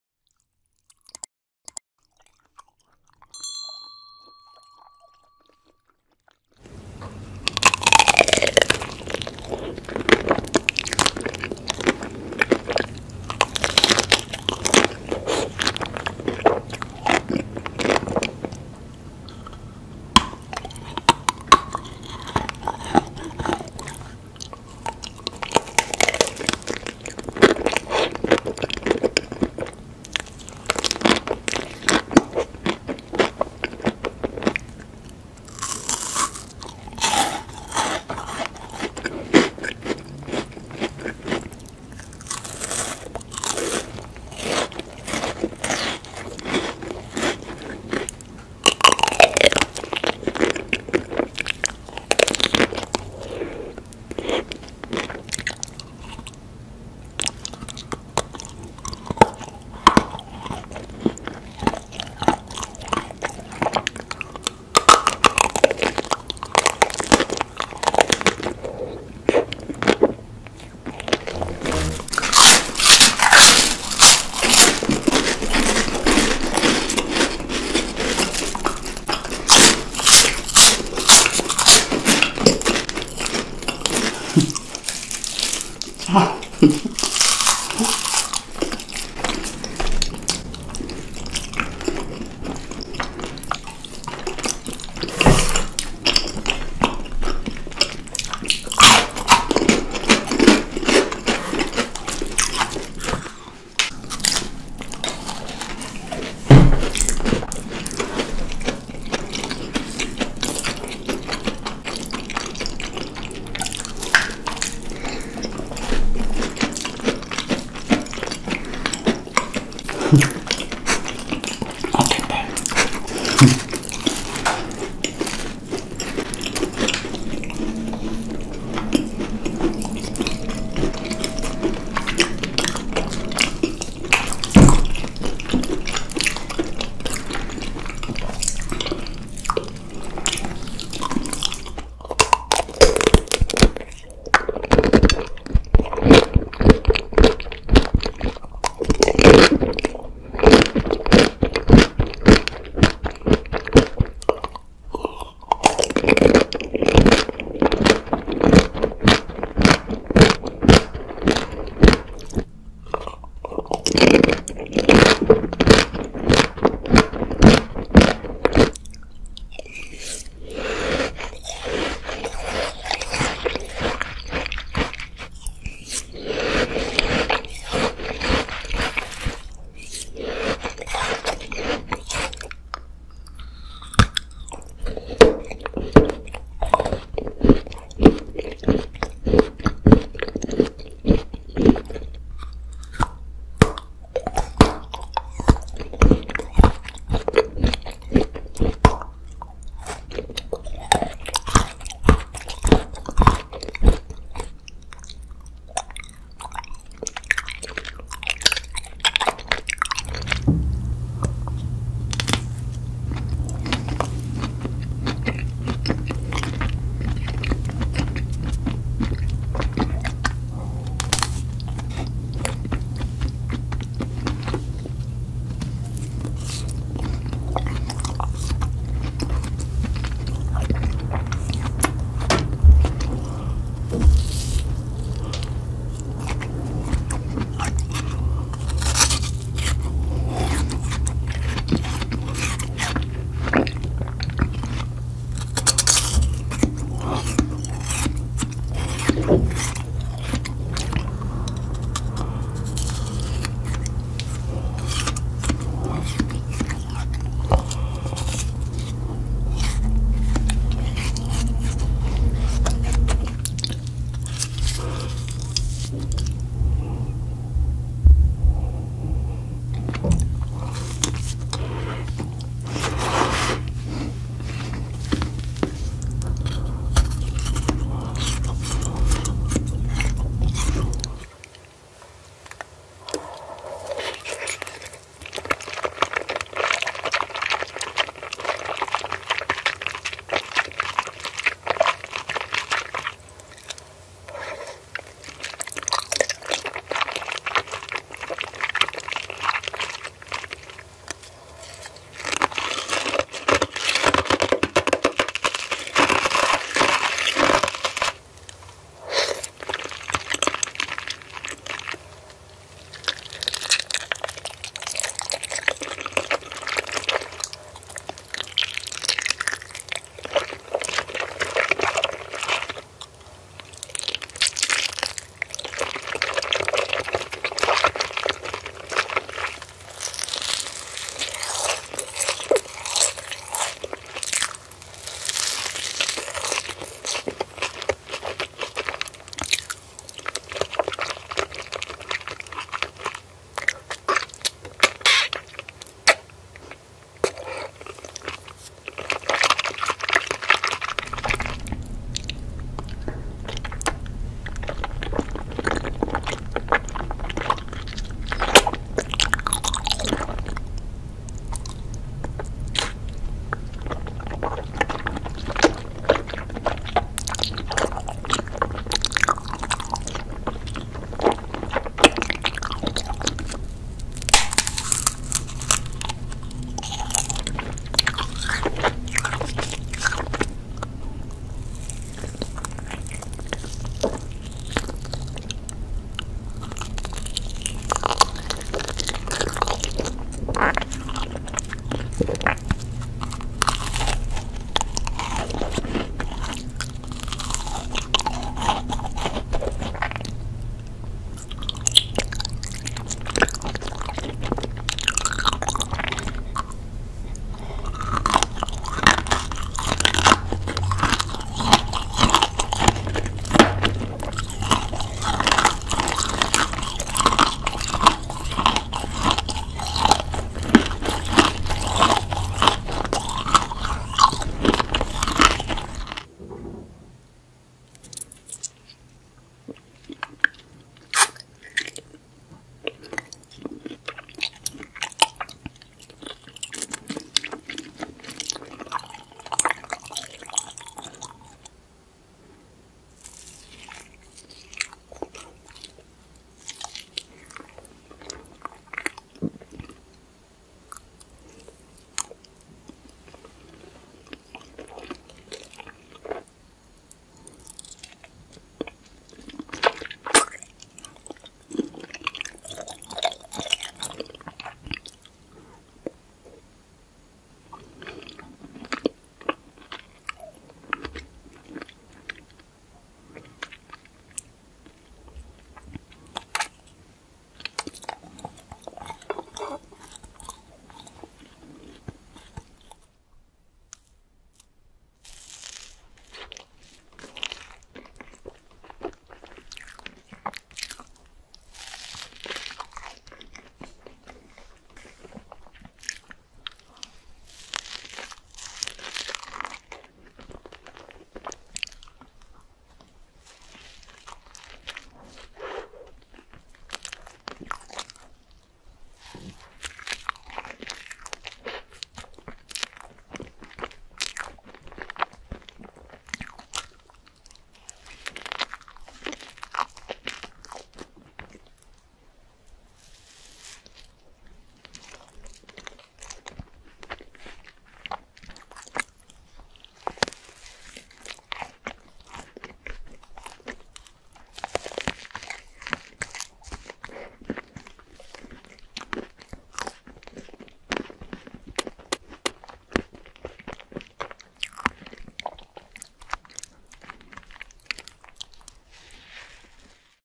Best of Asmr eating compilation - HunniBee, Jane, Kim and Liz, Abbey, Hongyu ASMR | ASMR PART 380
In this video you will see a compilation of the best moments of popular asmr eating bloggers